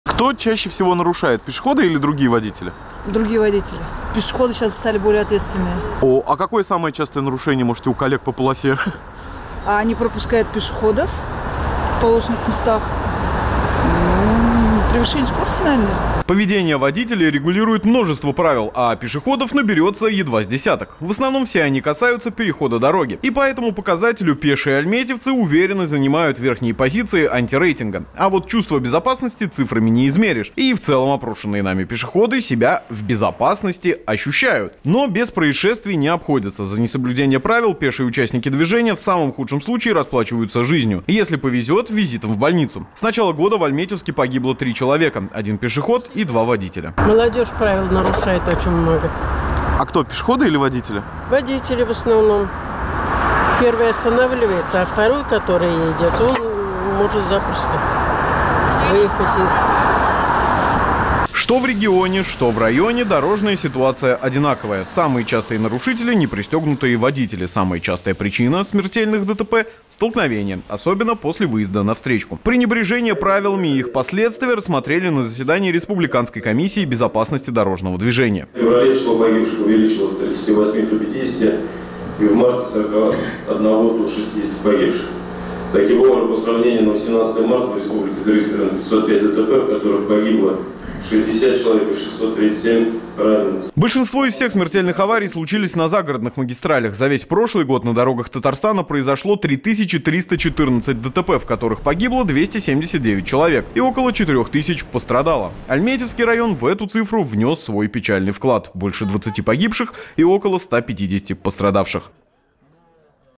Альметьевск вышел из антирейтинга по травматизму и смертности на дорогах
Альметьевск вышел из антирейтинга по травматизму и смертности на дорогах – если оценивать итоги двух месяцев. За это время погибших и пострадавших в 2024 меньше чем в прошлом году – такую информацию дают госавтоинспекторы. С начала года погибло три человека – 1 пешеход и два водителя.